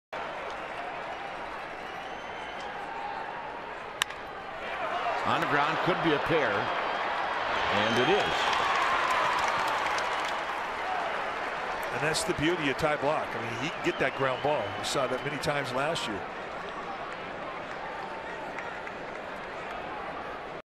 SF@ARI: Panik, Crawford turn a double play in the 8th
Joe Panik makes the stop on a grounder and tosses to Brandon Crawford to start a key 4-6-3 double play in the 8th inning